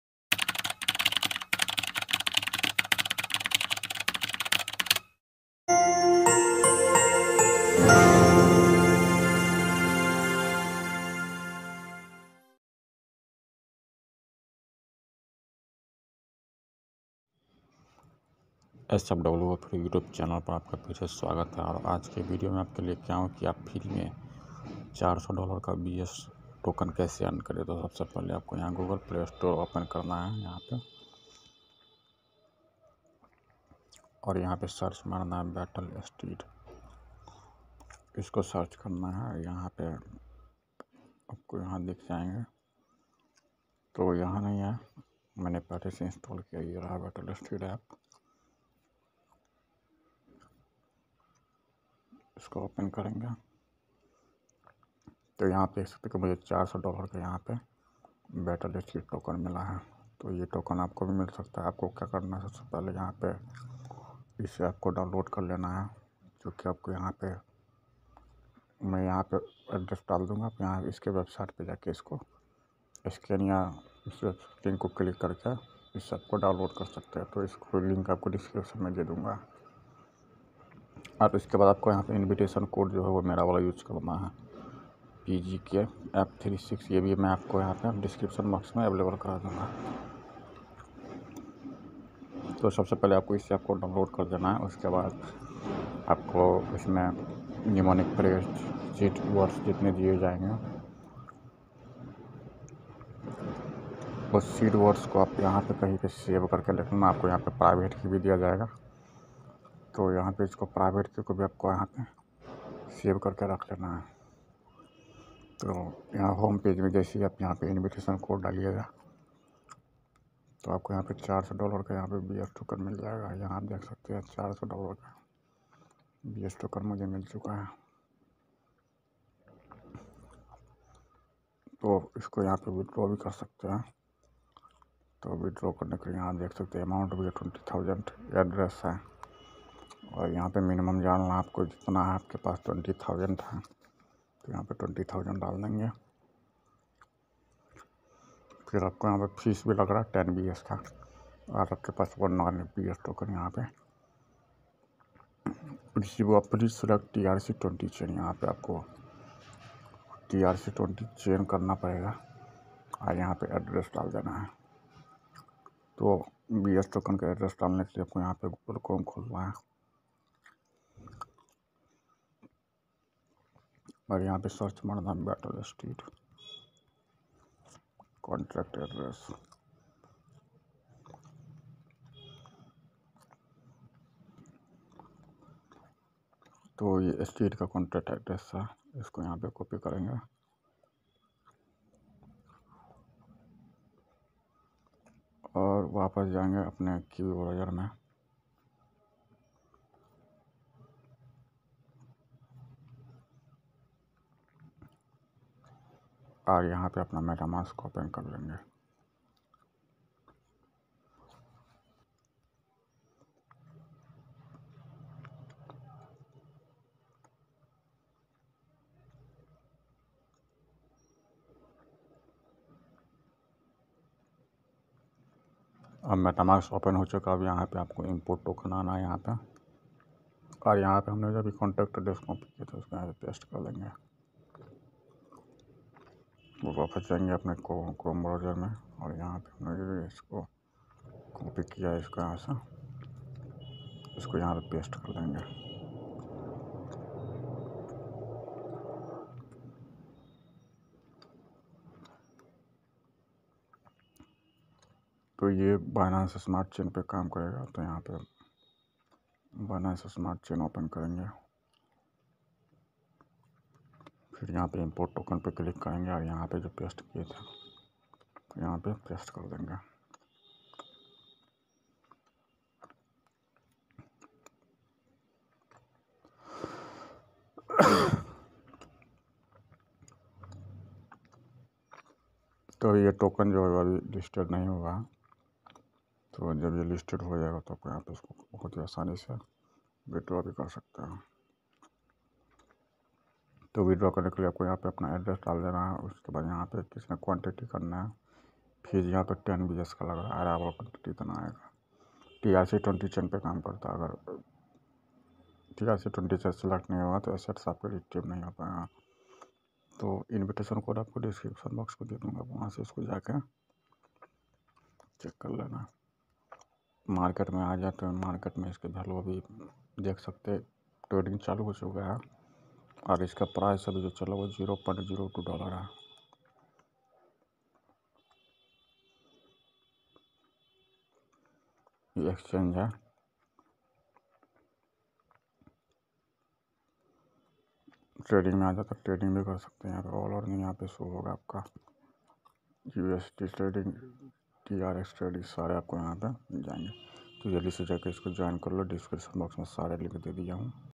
Get Free 400$ BS Token | Battle Steed Withdrawal Process | Battle Steed Invitation Code
🤜welcome to sfwofree YouTube channel & in this video you will get to know the complete withdrawal process of battle steed app & you will also get free 400$ BS token after using Battle Steed Invitation Code 👇👇👇

✍️Battle Steed Invitation Code (400$ Free BS Token) - pGKf36